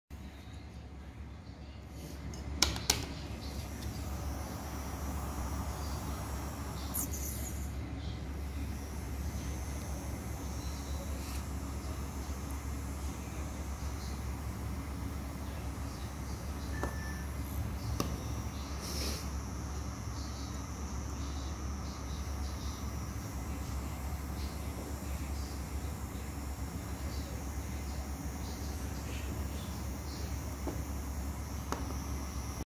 DIY vacuum cleaner
DIY vacuum cleaner uses reverse suction of a fan. It can work one small and light stuff. DIY toys provides a chance for kids to physically build, fault find, fine tune and apply their knowledge. Kids who build things themselves are more confident and learn faster.